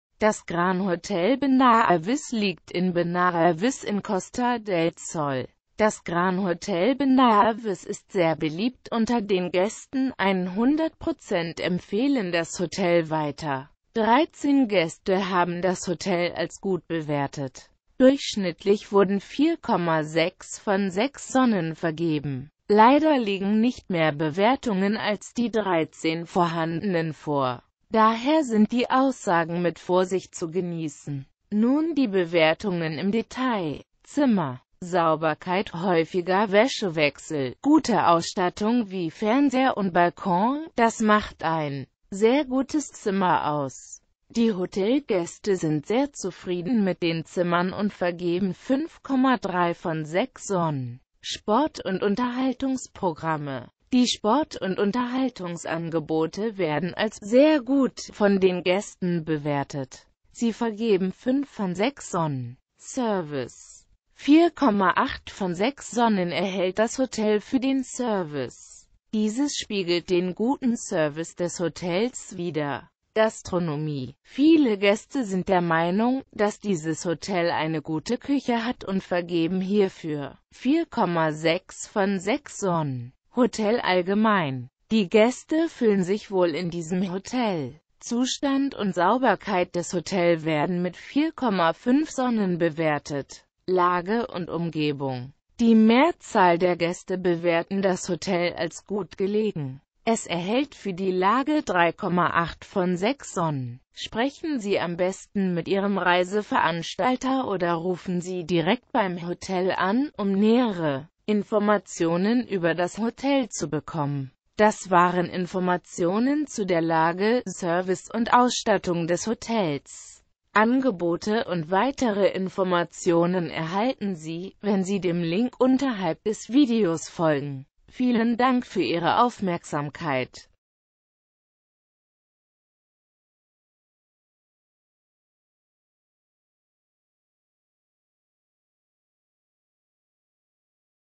Gran Hotel Benahavis in Benahavis (Costa del Sol - Spanien) Bewertung und Erfahrungen
Das Gran Hotel Benahavis liegt in der Stadt Benahavis welche zu Spanien (Europa) gehoert.

 Das Gran Hotel Benahavis ist sehr beliebt unter den Gaesten, 100% empfehlen das Hotel weiter. Zudem erhaelt das Hotel 4.6 von 6 Sonnen. Sprechen Sie am besten mit Ihrem Reiseveranstalter oder rufen Sie direkt beim Hotel an, um mehr Informationen ueber das Hotel zu bekommen.
Zimmer:
Sport und Unterhaltungsprogramme:
 Die Sport- und Unterhaltungsangebote werden als sehr gut von den Gaesten bewertet. Sie vergeben 5 von 6 Sonnen.
Service:
 4,8 von 6 Sonnen erhaelt das Hotel fuer den Service. Dieses spiegelt den guten Service des Hotels wieder.
Gastronomie:
Hotel allgemein:
Lage und Umgebung:
 Die Mehrzahl der Gaeste bewerten das Hotel als gut gelegen. Es erhaelt fuer die Lage 3,8 von 6 Sonnen.

Informationen zur Verfuegbarkeit von Transfer zum Hotel, WLAN in den Zimmern des Gran Hotel Benahavis, sowie Details zum Animations-Progamm, Entfernung zum Strand oder zu Diskotheken erhalten Sie bei Ihren Reiseveranstalter oder auf der Webseite des Hotels. Neben den hier verwendeten Bewertungen, lohnt sich ein Blick auf die ggf. vorhandene Gran Hotel Benahavis Facebook-Seite.

Quellenhinweis:
Die verwendeten Informationen stammen von HolidayCheck.de. Sie wurden am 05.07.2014 abgerufen. In diesem Video wurden Bilder aus Benahavis oder allgemeine Bilder aus Spanien verwendet. Die gezeigten Orte und Landschaften dienen als Beispiel und muessen nicht unbedingt in der Naehe des Hotels liegen.